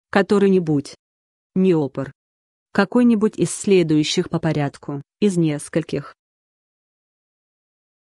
КОТОРЫЙ-НИБУДЬ - что это такое? значение и описание
Хочешь знать что значит КОТОРЫЙ-НИБУДЬ? тогда слушай до конца.

 мест. неопр. Какой-нибудь из следующих по порядку, из нескольких.